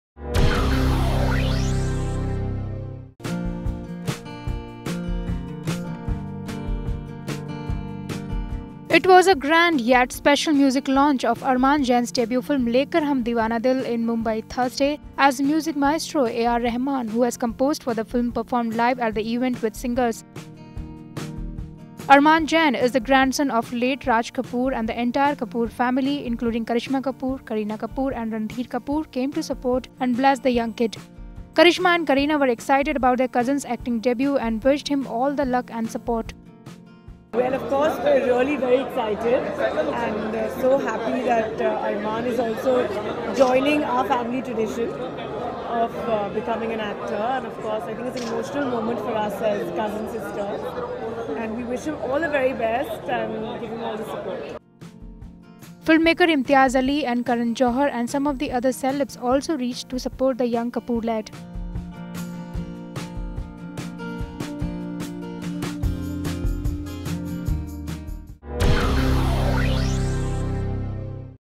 Karisma, Kareena support cousin Armaan Jain - Bollywood Country Videos
It was a grand yet special music launch of Armaan Jain's debut film 'Lekar Hum Deewana Dil', in Mumbai, Thursday. Armaan  is the grandson of late Raj Kapoor and the entire Kapoor family, including Karisma Kapoor, Kareena Kapoor and Randhir Kapoor came to support and bless the young kid.